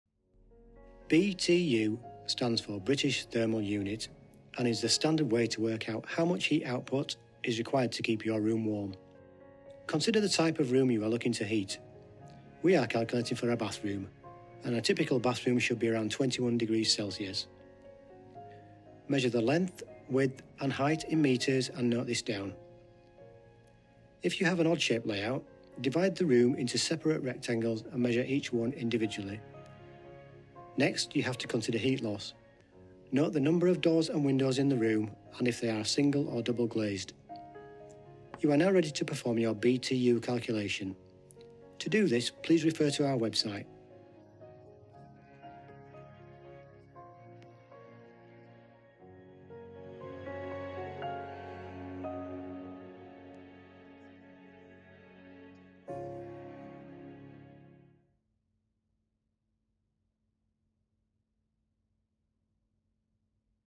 How to Calculate BTU in 60 Seconds!
Need the right size radiator or air conditioner? Learn how to calculate BTU (British Thermal Units) quickly and easily! We’ll cover:
- Room size measurements
- Factors like insulation and windows
- Simple BTU calculation formula